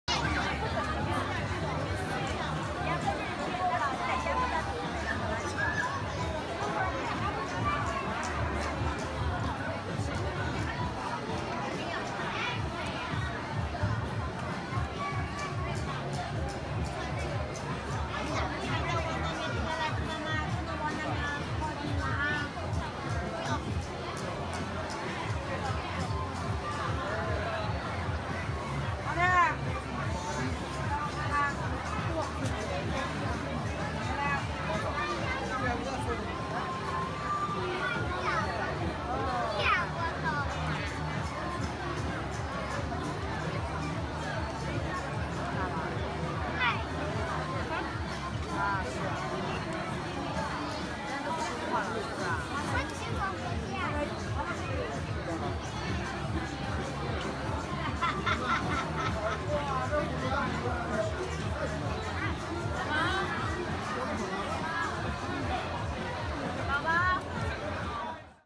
Sugar Handicraft, Qinghefang street in Hangzhou,CHN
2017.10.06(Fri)
Qinghefang Special History and Culture Street District in Hangzhou.

정말 먹어보고 싶었다.
여친님이 중국에서 아이들이 좋아하는 과자라고 말하기 전까지..
욕망에 불타는 내 눈동자를 읽은 여친님의 선공에 구경만 할뿐 ㅠㅠ